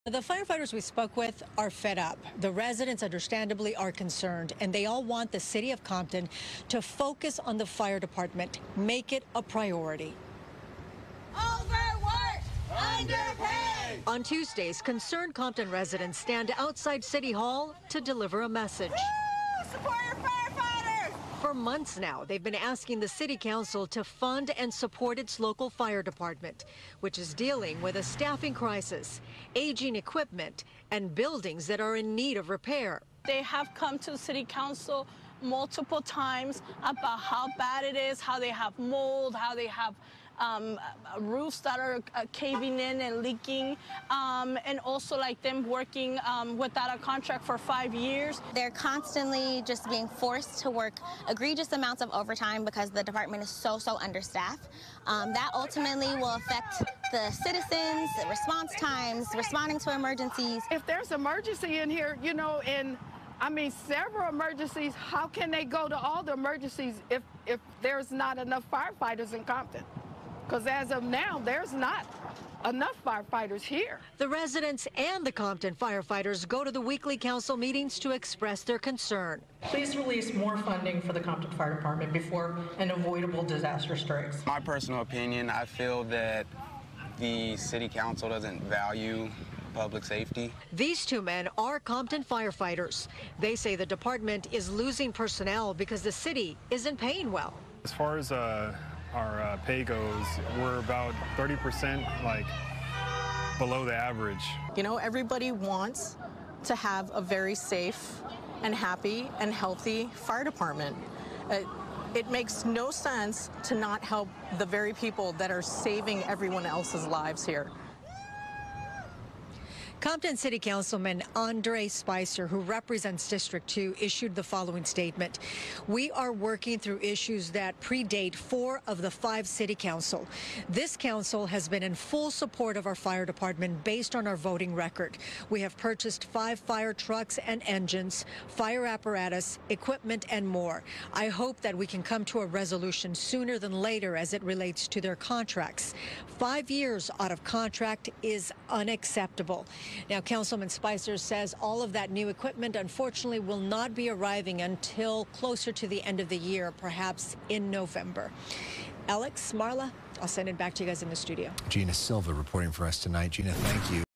Compton residents fight for fire dept.
Residents in Compton are standing up for the city fire department as they are pushing for more funds and support for the crew members.